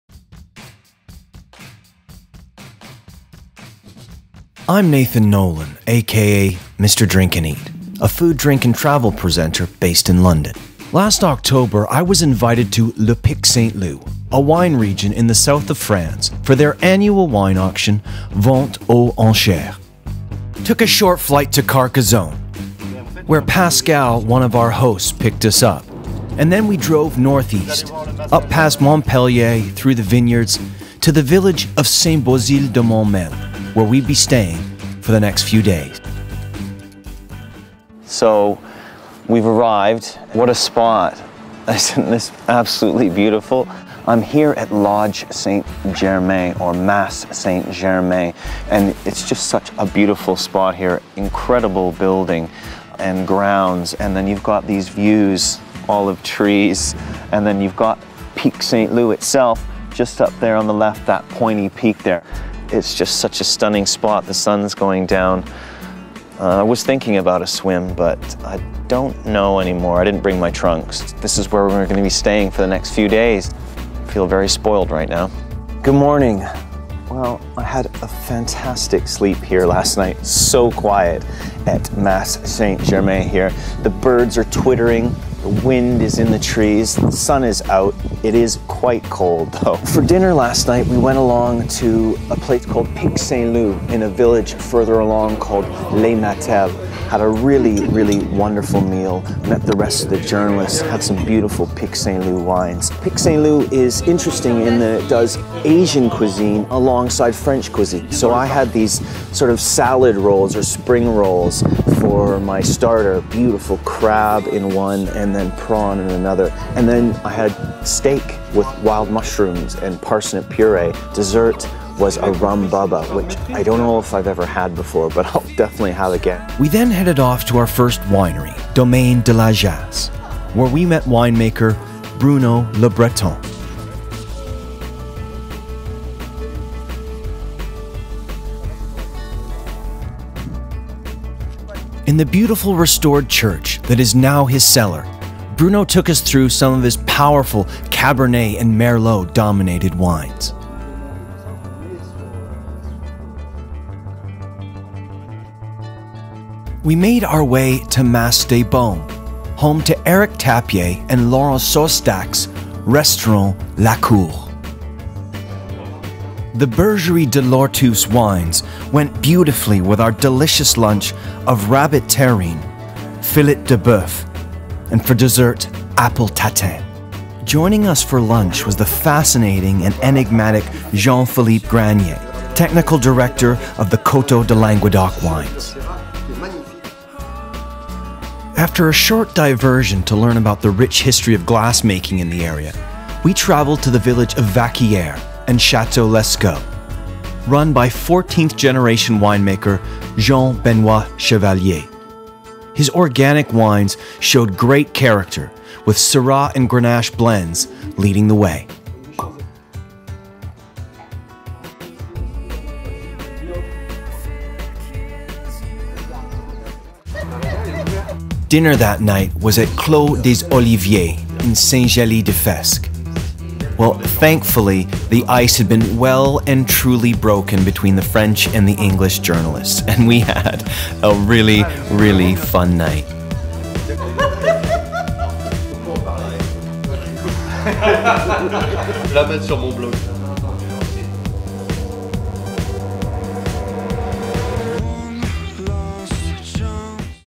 Pic St Loup: Part 1
My 1st visit to the lovely southern French wine region Le Pic St Loup. The food, the wine were as wonderful and dynamic as the people I met. On my first few days I ate at the wonderful  Le Pic St Loup Restaurant & Restaurant Le Cour. I also visited the wineries of Domaine de la Jasse and Chateau Lascaux. More to come in part 2.